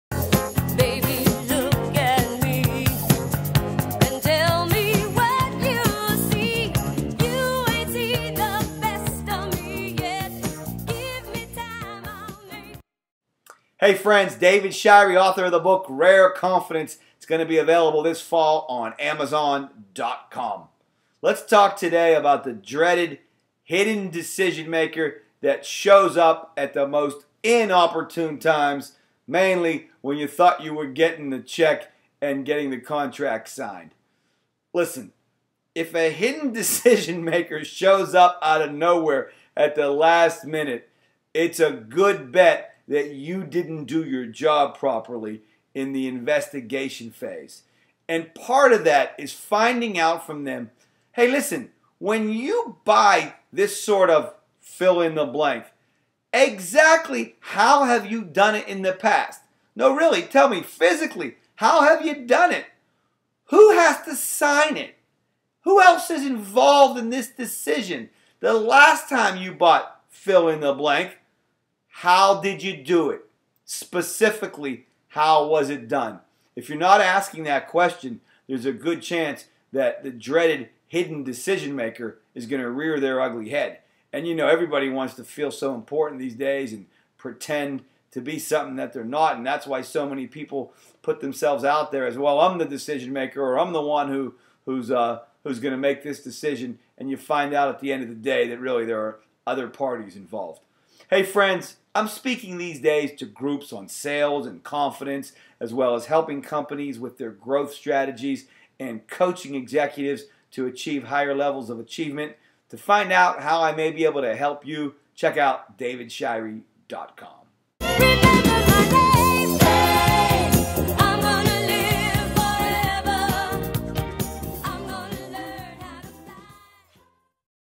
The Hidden Decision Maker
David Shirey, Sales & Confidence Success Coach, warns you about the dreaded hidden decision maker. He (or she) will kill the deal every time if not identified early enough in the process!